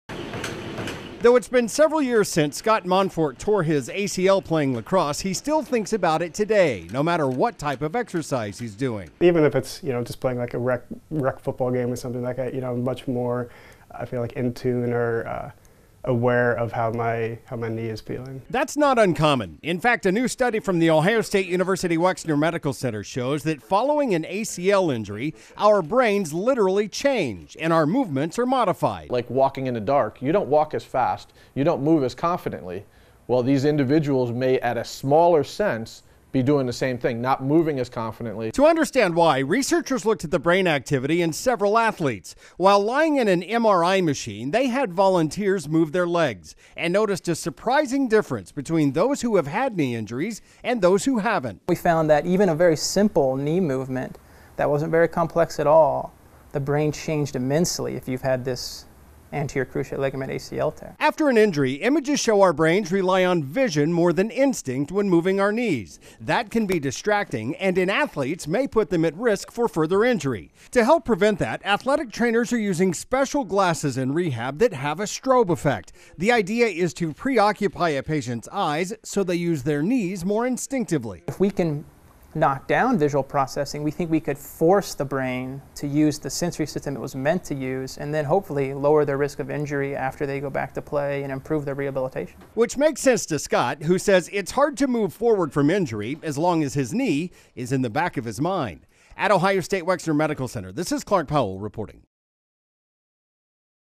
Your Brain Impacts ACL Surgery Recovery
Discover how Ohio State research is shedding light on our mind’s role in ACL surgery recovery.

Recovering from an ACL injury is a hard battle to fight, and it’s not just physical. Researchers at The Ohio State University Wexner Medical Center found that coming back from knee trauma requires retraining the brain to aid in physical therapy.